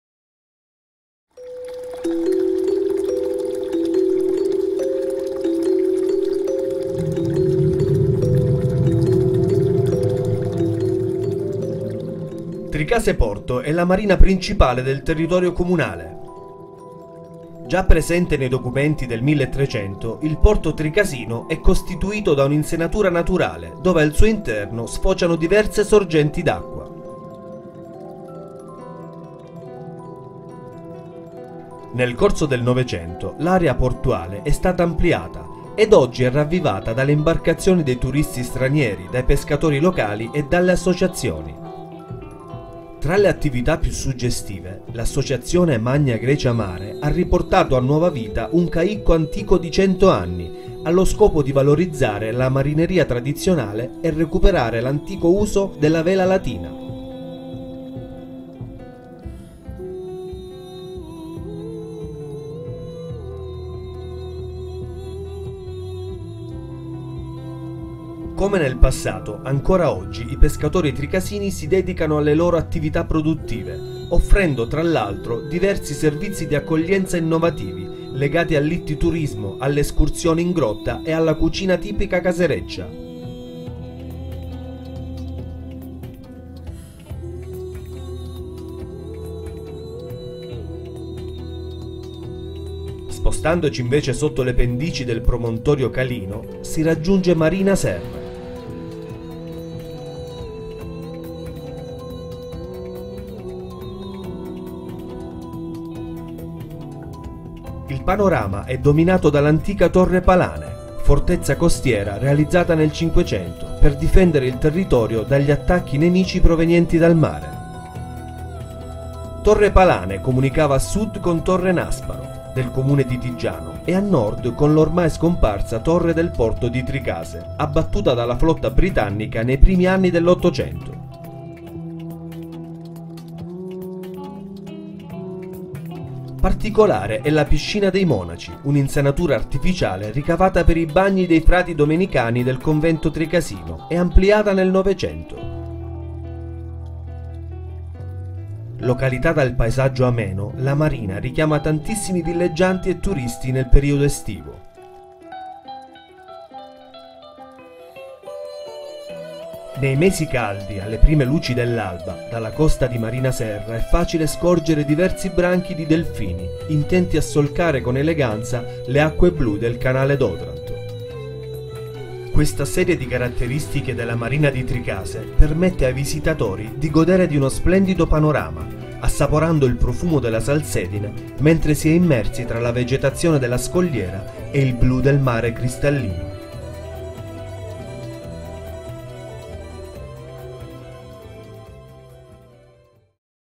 Città di Tricase - Tricase Porto e Marina Serra
Tricase Porto è la marina principale del territorio comunale.
Già presente nei documenti del 1300, il porto tricasino è costituito da un’insenatura naturale dove al suo interno sfociano diverse sorgenti d’acqua.
Nel corso del Novecento l’area portuale è stata ampliata ed oggi è ravvivata dalle imbarcazioni dei turisti stranieri, dai pescatori locali e dalle associazioni.
Tra le attività più suggestive, l'Associazione Magna Grecia Mare ha riportato a nuova vita un caicco antico di 100 anni, allo scopo di valorizzare la marineria tradizionale e recuperare l’antico uso della vela latina.